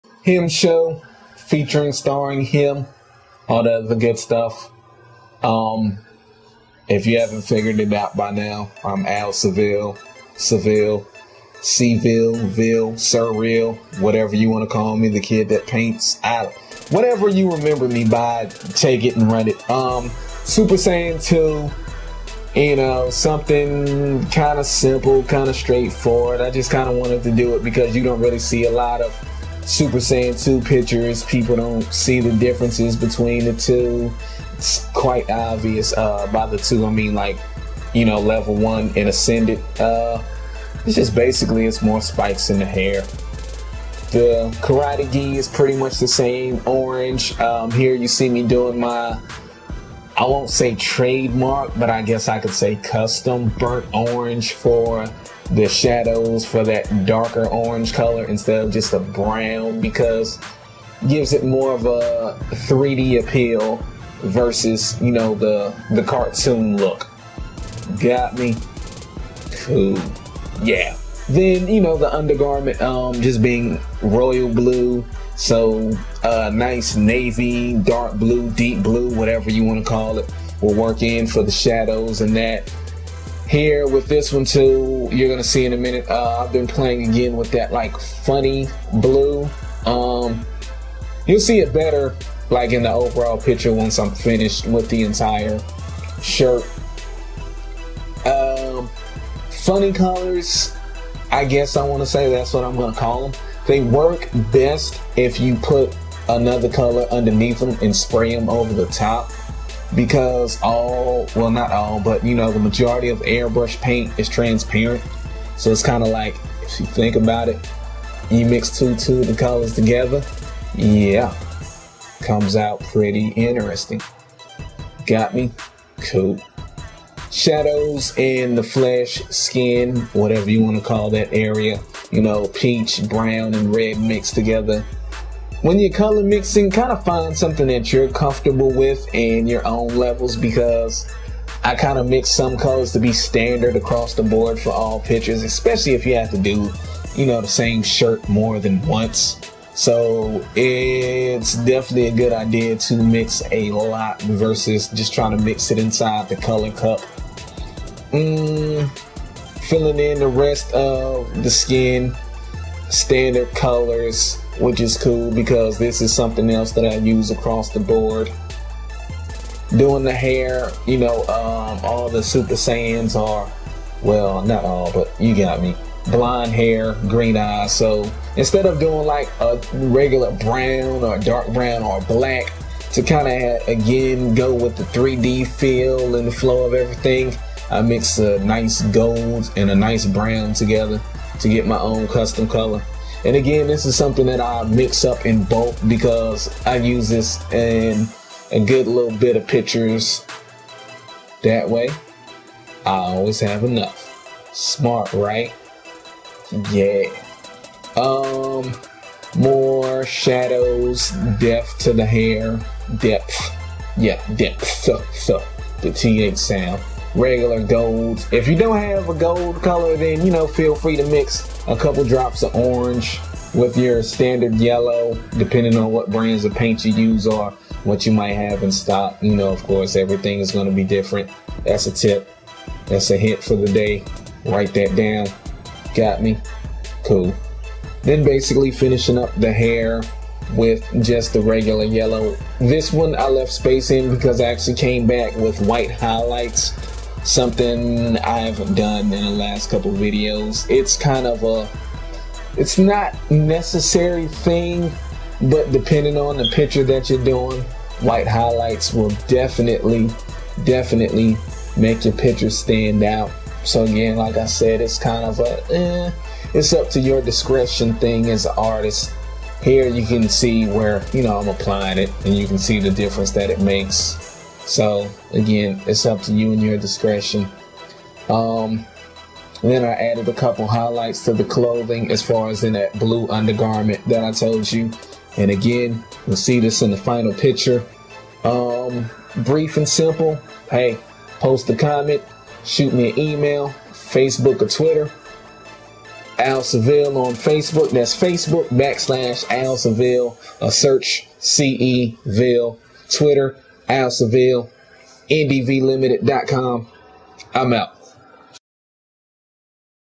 LIVE AIRBURSHING: TIPS & TECHNIQUES!! SSJ2 GOKU (DRAGON BALL Z)
ASCENDED!! Since you RARELY ever see this transformation (and I'm a completionist) I figured Id round out the circle and airbrush SSJ2 Goku. And yeah I'm that big of an anime fan. V__(^__^)__V  Check the white highlights on this one. They really make a difference when you want to emphasize brightness, lighting effects, glow(s) etc.